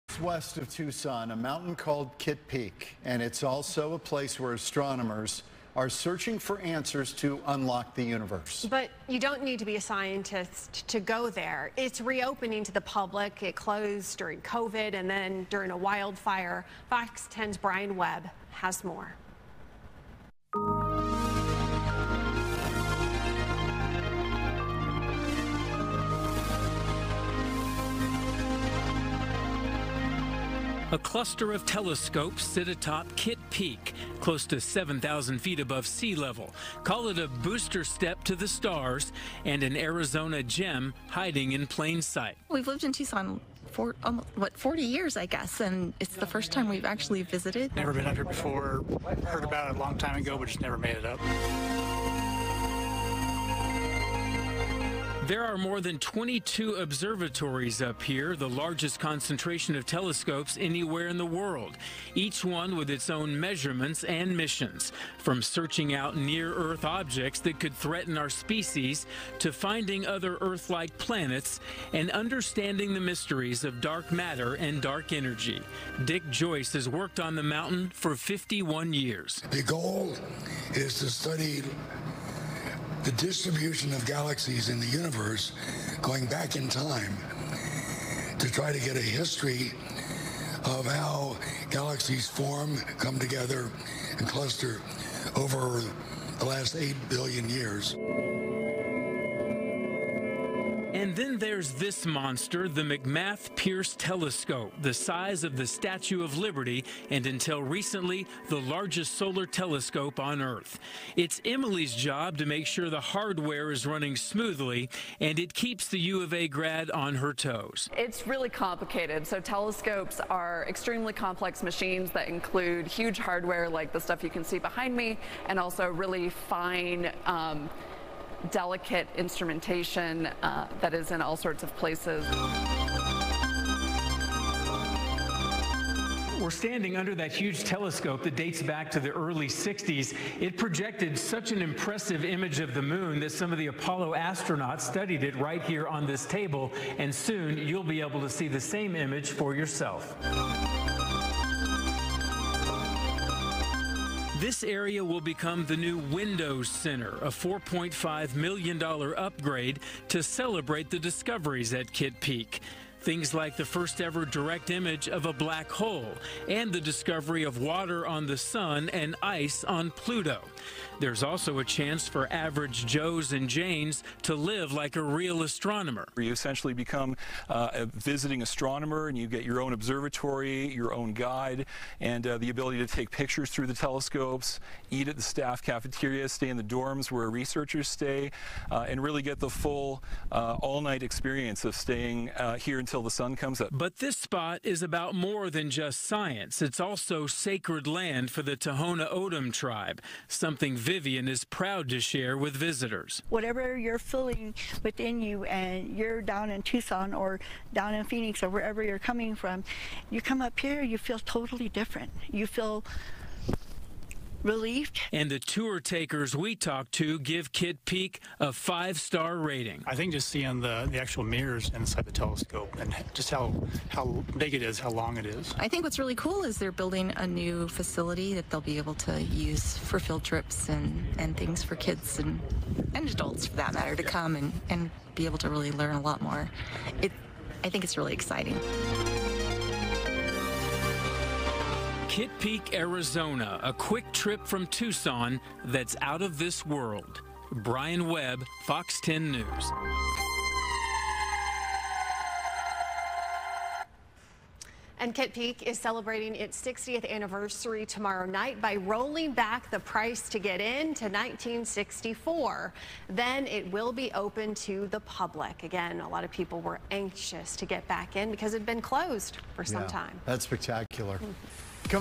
Kitt Peak National Observatory is reopening
Home to one of the largest telescopes in the world, Kitt Peak National Observatory, just west of Tucson, is reopening to the public after a lengthy closure due to COVID and a wildfire. FOX 10's Brian Webb has more.